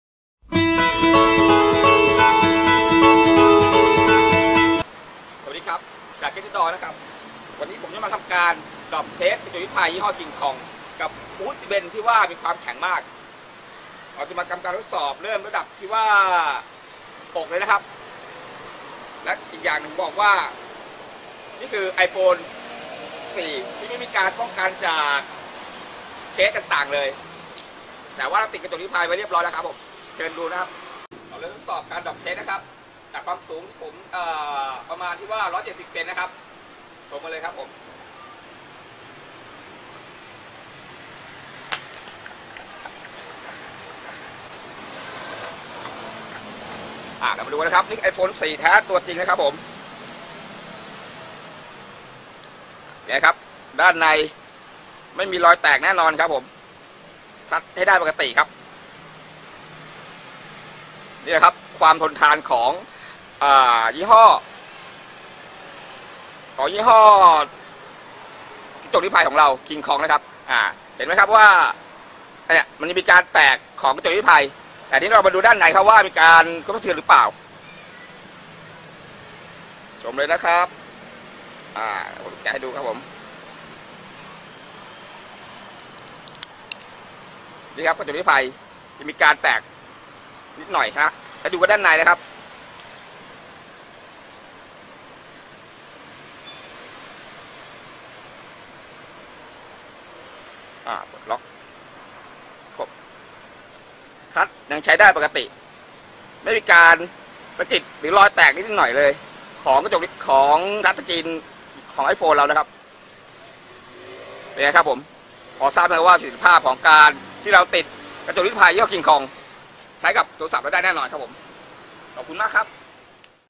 การทดสอบกระจกนิรภัยking kong (การตกจากที่สูงบนพื้นปูน)
กระจกนิรภัย kingkong เป็นกระจกชนิดพิเศษที่ถูกสร้างขึ้นมาเพื่อ­ปกป้องหน้าจอโทรศัพท์มือถือหรือแท็บเล็ตสำ­หรับใช้แทนฟิล์มกันรอยทั่วไป เป็นระบบสูญญากาศ หายห่วงเรื่องคราบกาวติดหน้าจอ กันรอยขีดข่วน ลดรอยนิ้วมือ และกันกระแทกจากหน้าจอ เช็ดออกง่าย หน้าจอใสเหมือนทัชจากหน้าจอจริง รวมถึงปกป้องจอของคุณเมื่อหล่นพื้น